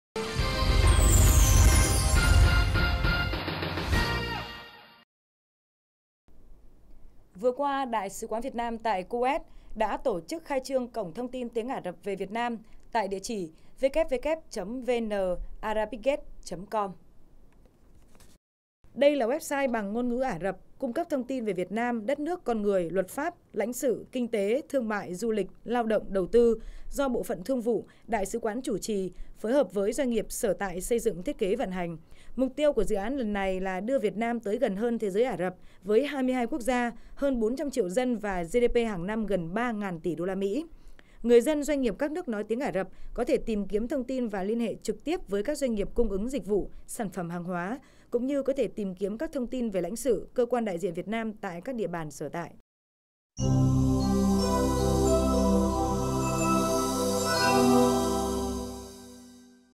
Khai trương Cổng thông tin tiếng Ả Rập về Việt Nam
Vừa qua, Đại sứ quán Việt Nam tại Kuwait đã tổ chức khai trương Cổng thông tin tiếng Ả Rập về Việt Nam tại địa chỉ